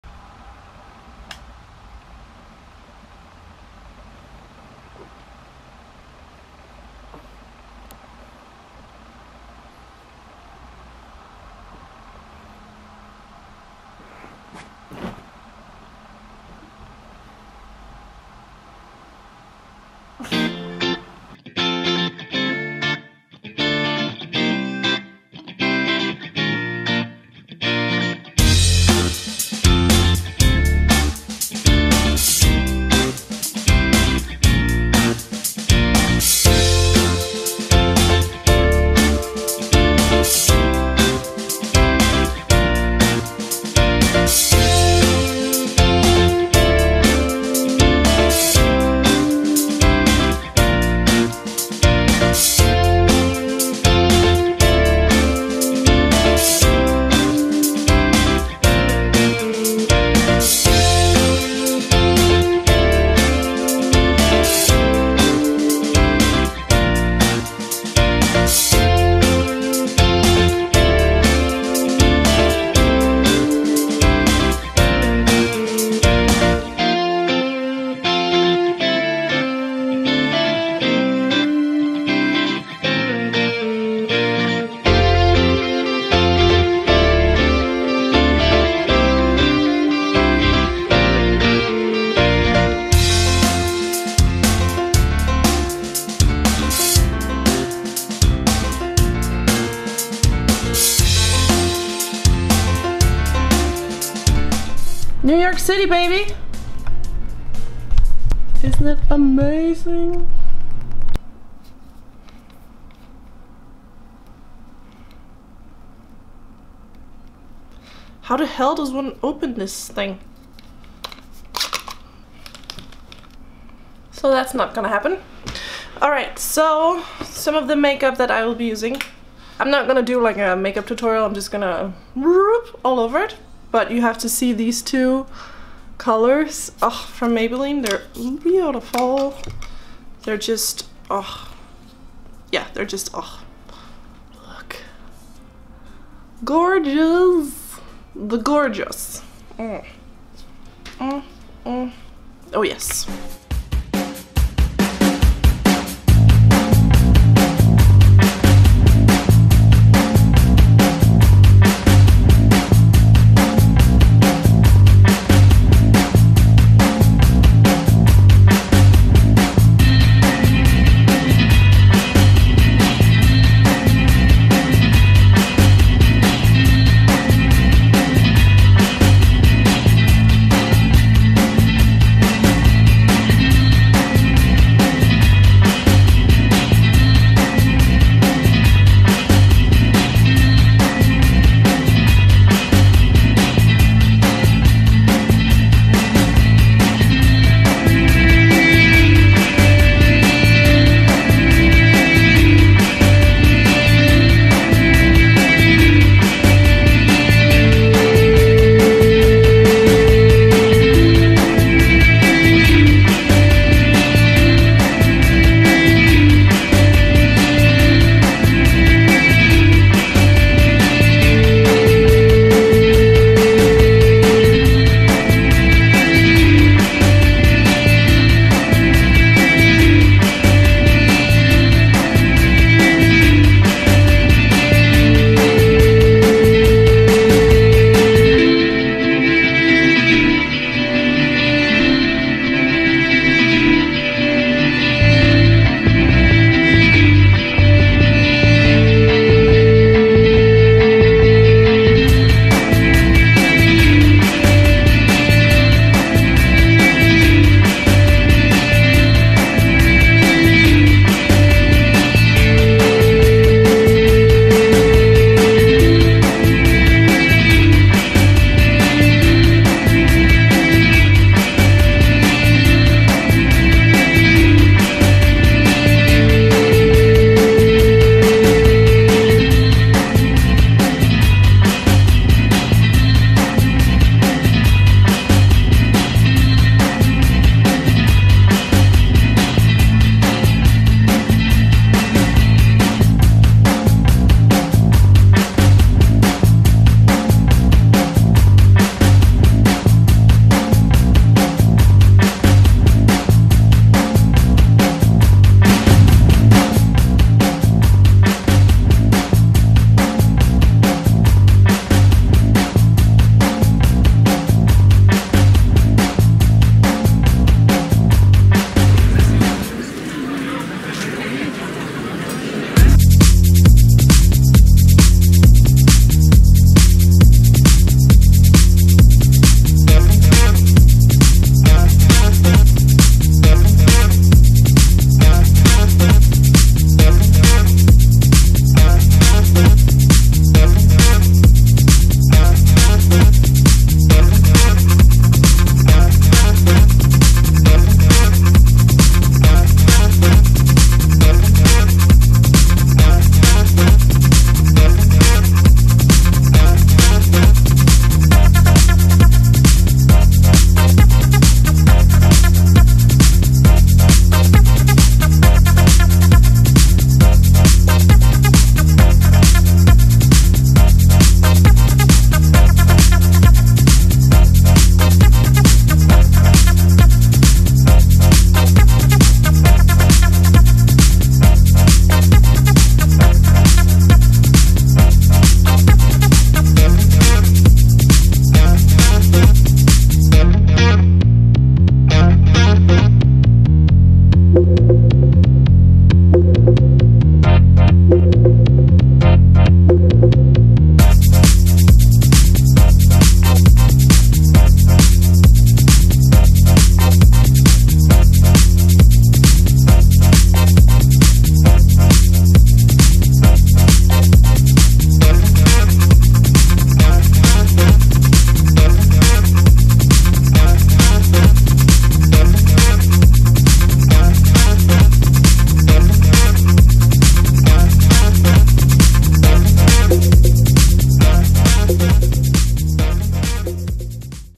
VLOG 1 | COME WITH ME TO NEW YORK FASHIONWEEK
My very first VLOG! I hope you guys like this video - it is about my trip to NYC during fashionweek together with Essie and Maybelline.

Say hi to me on facebook!